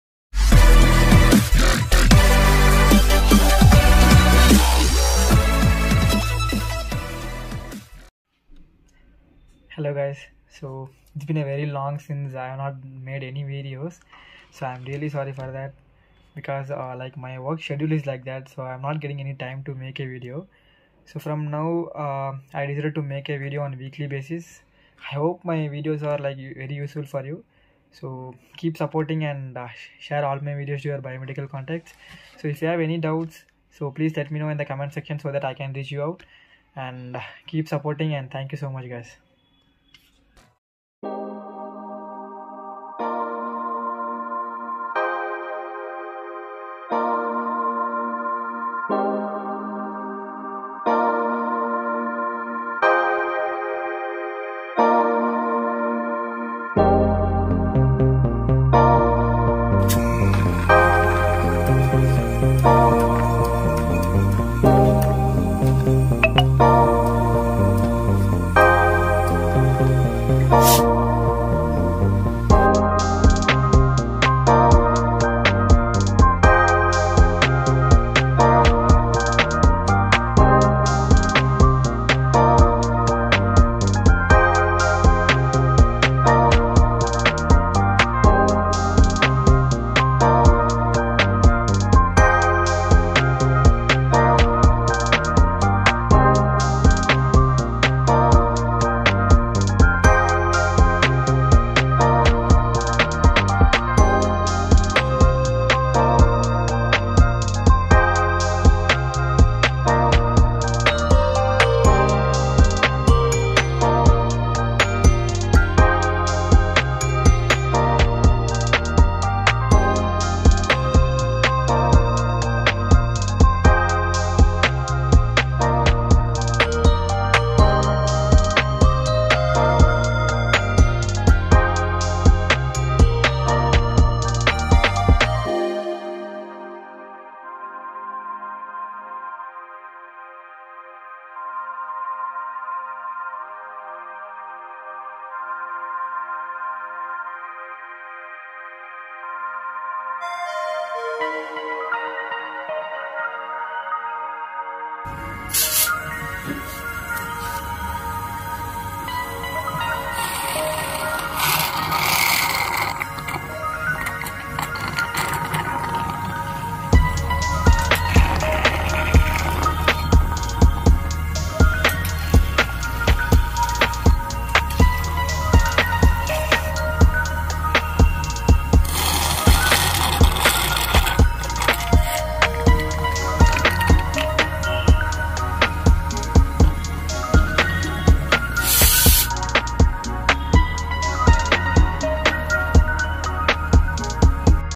"Suction Failure Resolved – Dental Chair Running Smoothly Again! 🦷🔧"
Equipment name: Dental Chair
Make: Gnatus S200
Model: S200

"Welcome to Biomedical Diaries! Today, we tackle one of the most frustrating dental equipment challenges – SUCTION FAILURE. 🛑🦷 But no worries, we've got it resolved! In this episode, we’ll walk you through the diagnostic process and share expert-level tips to bring your dental chair suction system back to life.

Get ready for a hands-on breakdown, covering common causes, quick fixes, and ways to prevent this issue from happening again. If you're into biomedical maintenance, repair tricks, or just love diving into the tech side of healthcare, this one’s for you!

Don't let small issues stop you – let's keep things running smoothly! ⚙️🚀